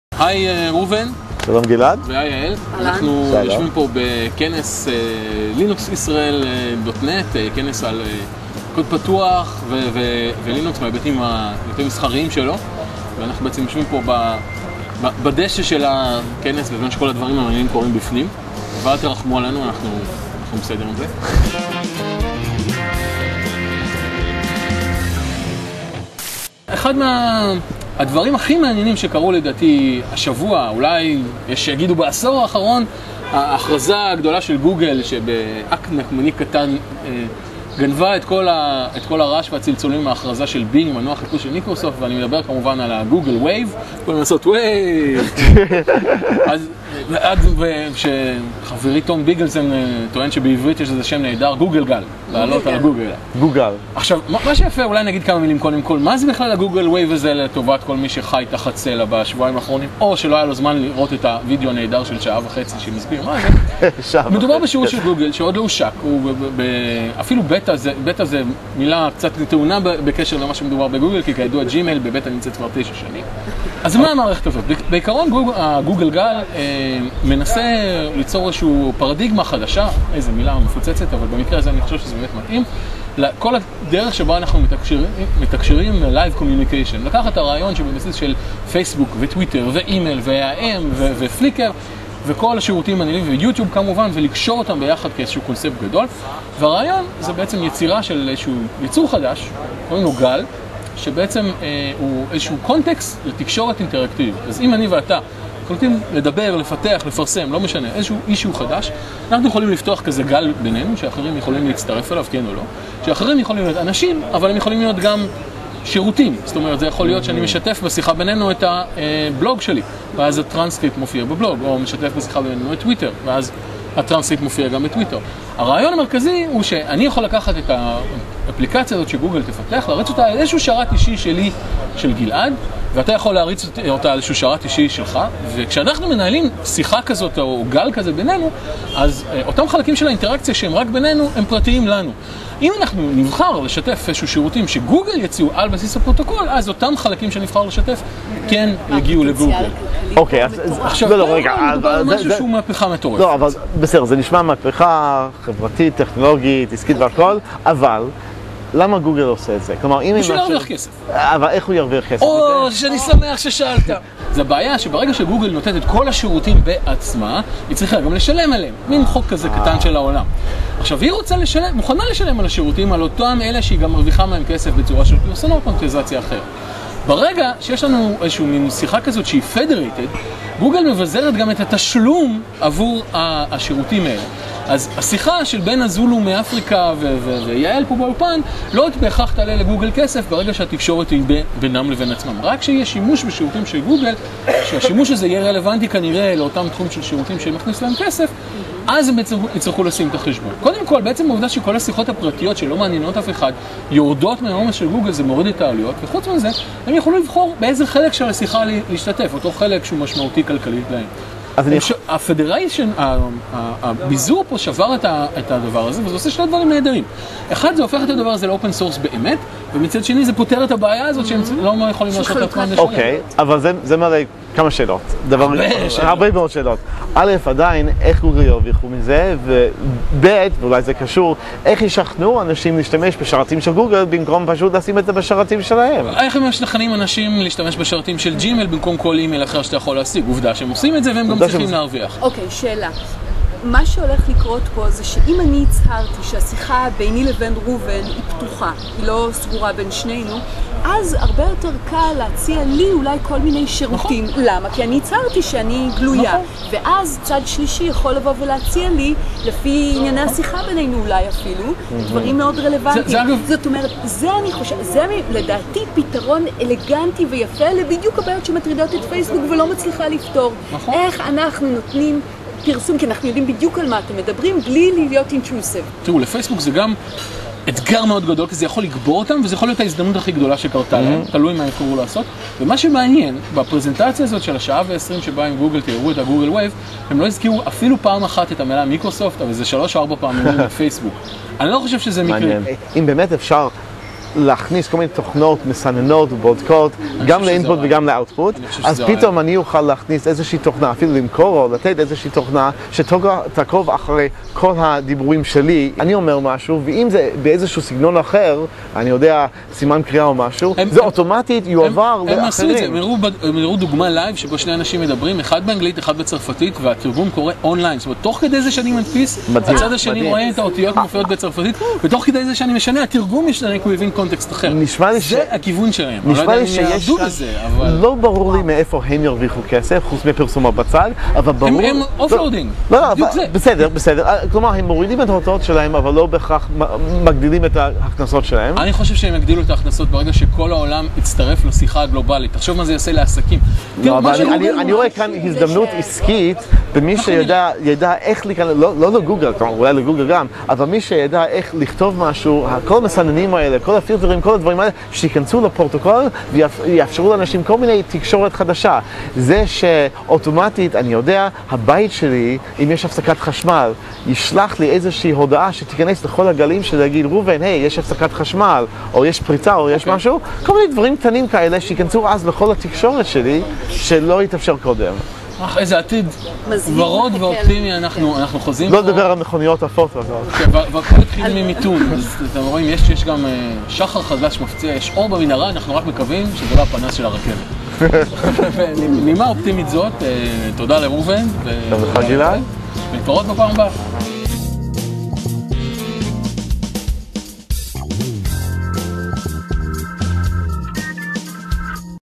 R&G Show: Google Wave
Reuven & Gilad meet up at the Linux Israel 2009 conference to speak about Google Wave and some of the implications this new invention may have over our lives. They invite Yael to join them. Is Google Wave a threat to Facebook or an opportunity? What kind of new business opportunities will it create? What does Google expect to gain from the Wave?

For an introduction in Hebrew to the show please read the following